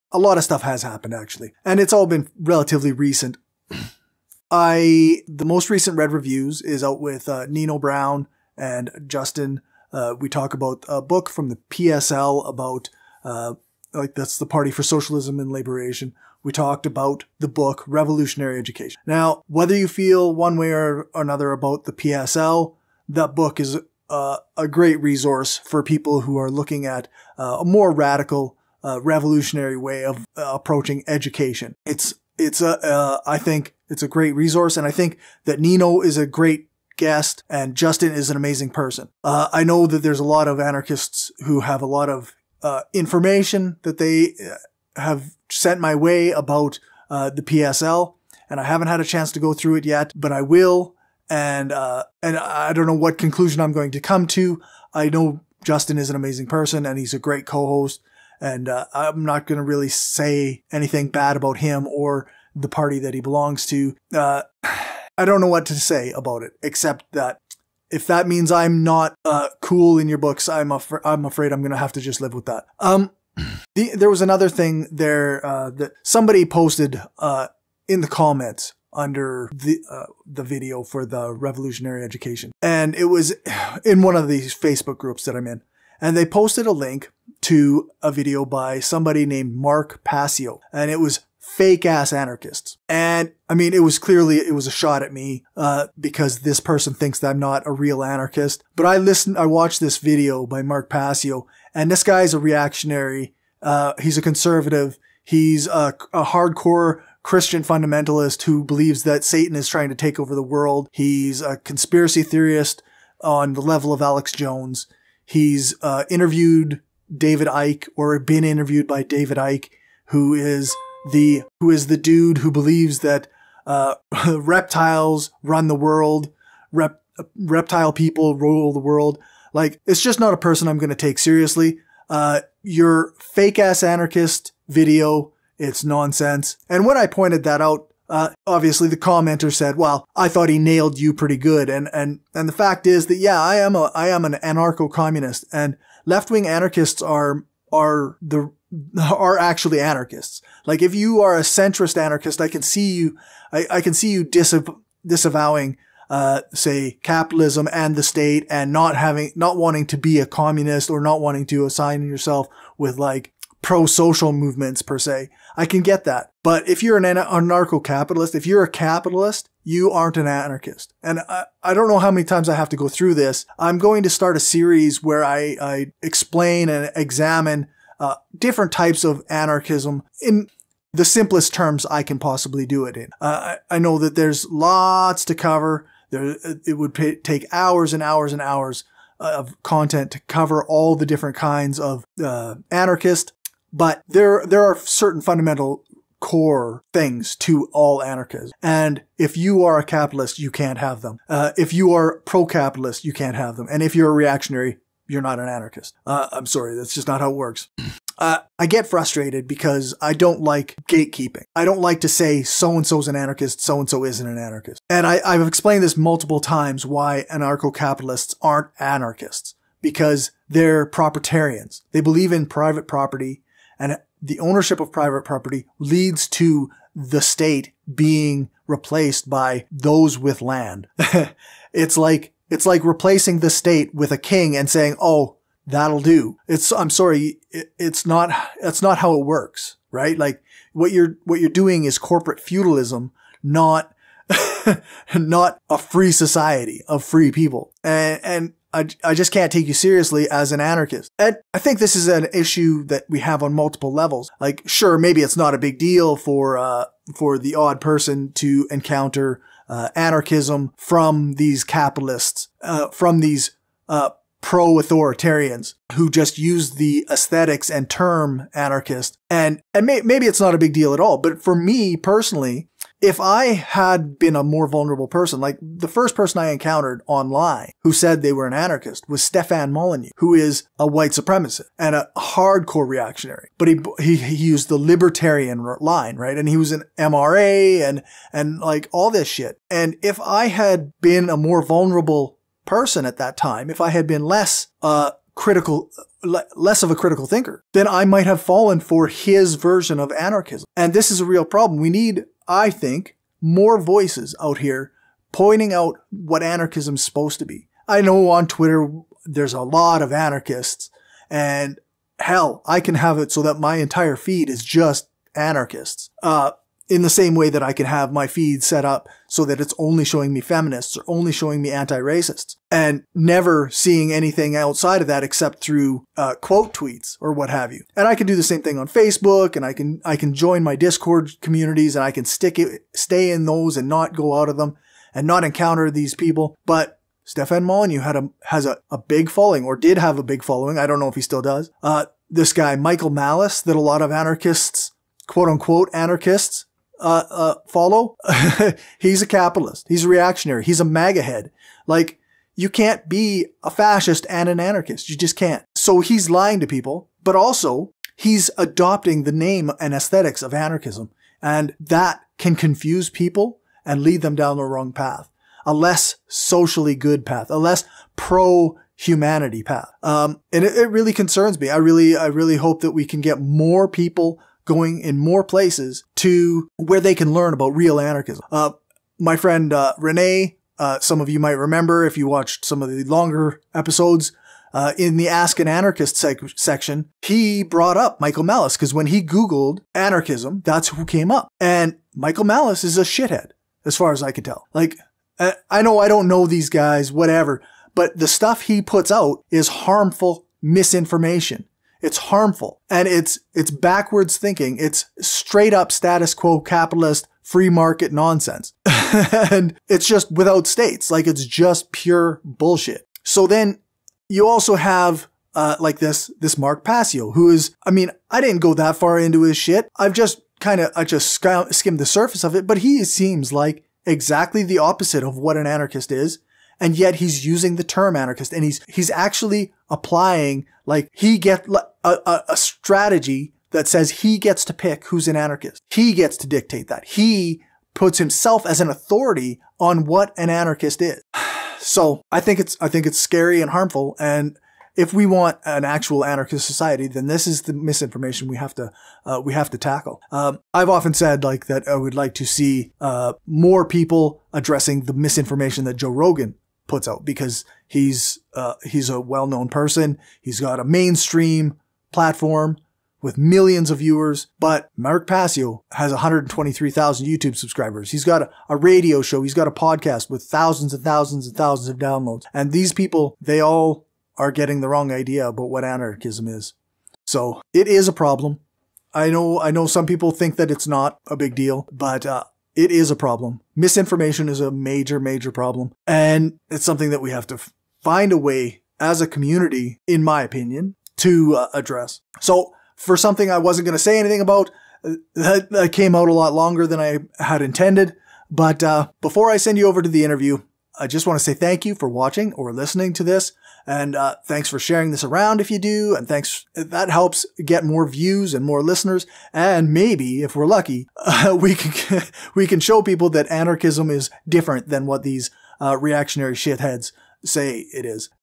Reactionaries Calling Themselves Anarchists
This was the intro from interview #49 with Tori Douglass